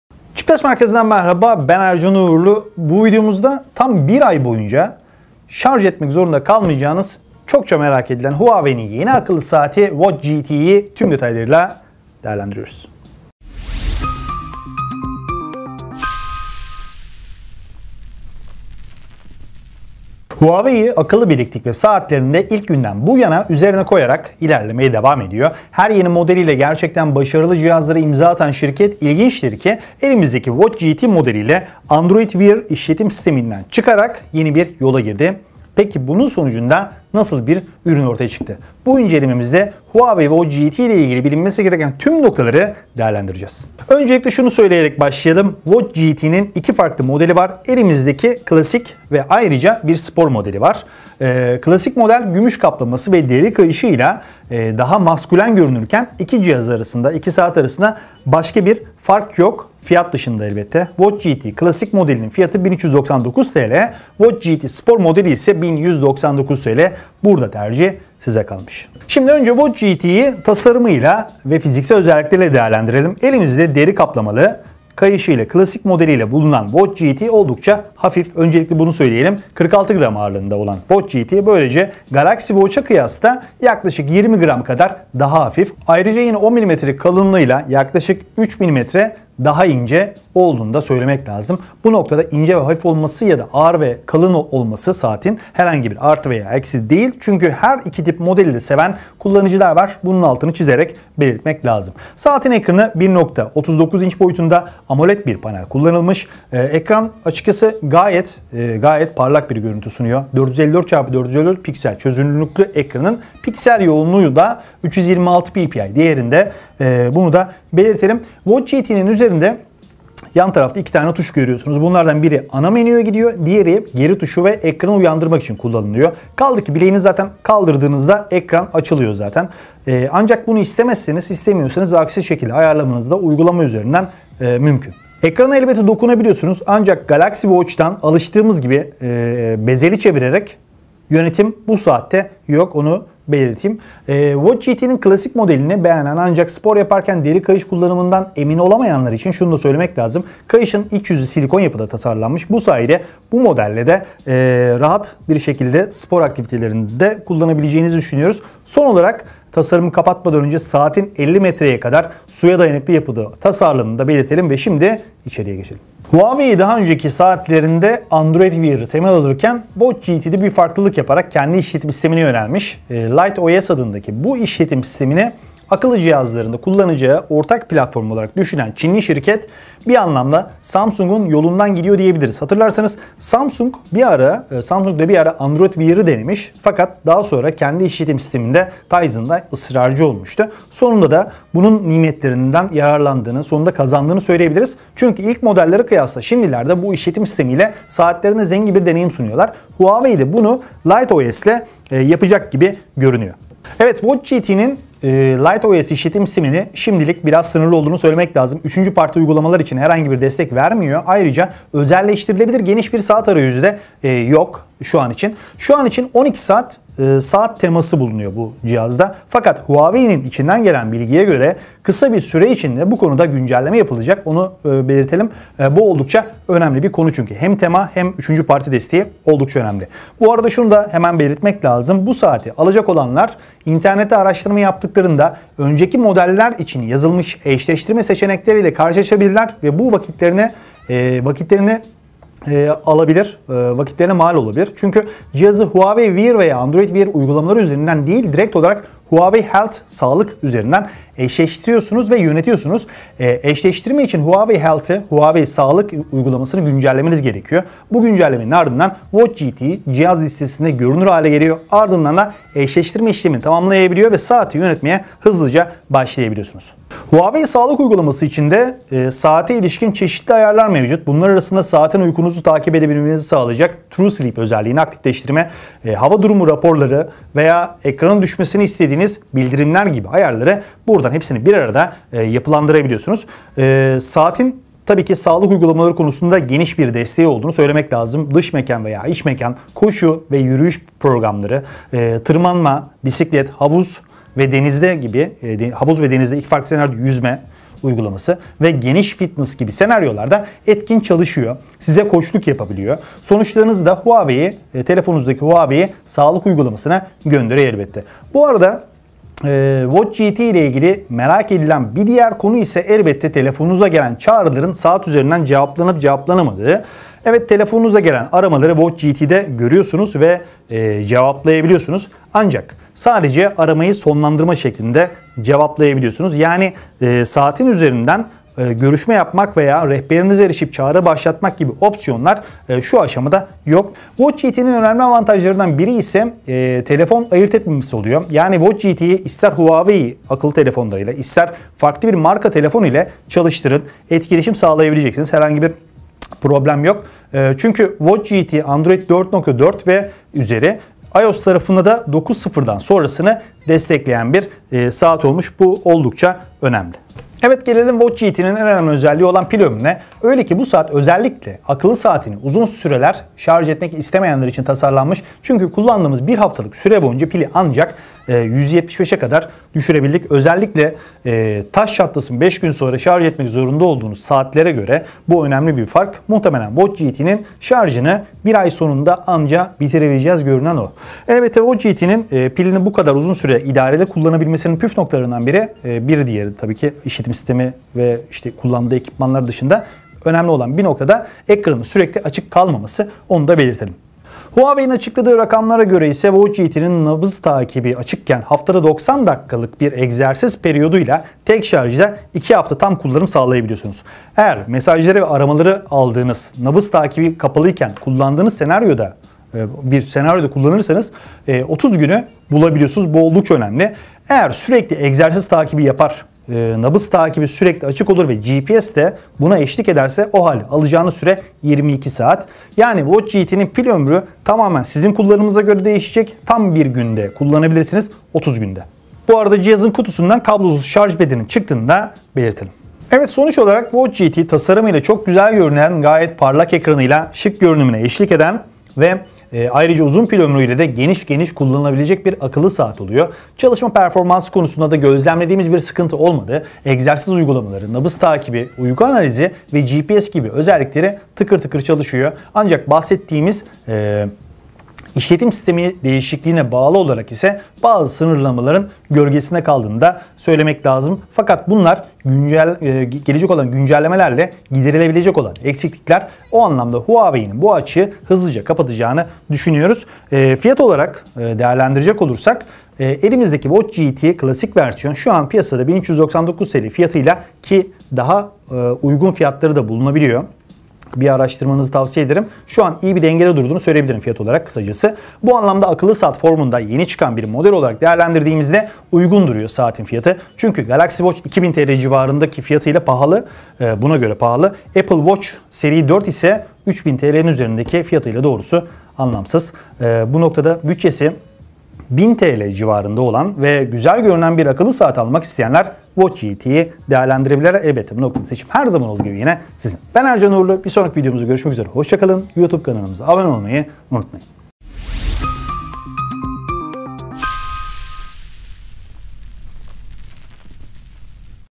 Huawei Watch GT İnceleme
Huawei, akıllı bileklik ve saatlerinde ilk günden bu yana üzerine koyarak ilerlemeye devam ediyor. Her yeni modeliyle gerçekten başarılı cihazlara imza atan şirket, ilginçtir ki, elimizdeki Watch GT modeliyle Android Wear işletim sisteminden çıkarak, yeni bir yola girdi. Peki, bunun sonucunda nasıl bir ürün ortaya çıktı? Bu incelememizde Huawei Watch GT ile ilgili bilinmesi gereken tüm noktaları değerlendireceğiz.

Ayrıca hiç beklenmedik zamanlarda ilginç vLog'lar da karşınıza çıkabilir!